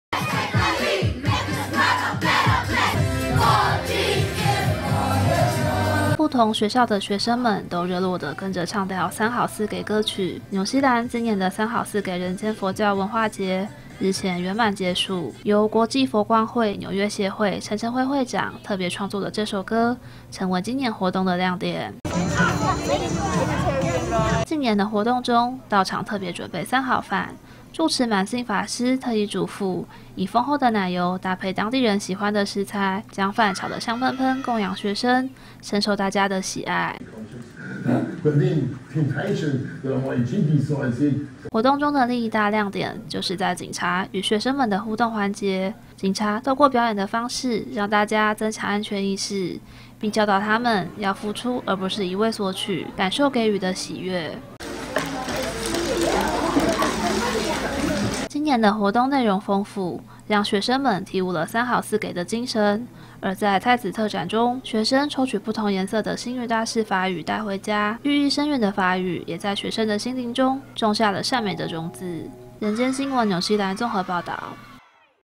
20240612 紐西蘭三好四給文化節 歷經一個月圓滿結束｜@FGSBLTV
落實佛光山開山祖師星雲大師「寺院學校化」的理念，今年紐西蘭佛光山第十屆"三好四給人間佛教文化節"共有32場活動，走遍南北傳遞三好四給理念，歷經了一個月，終於畫上圓滿句號。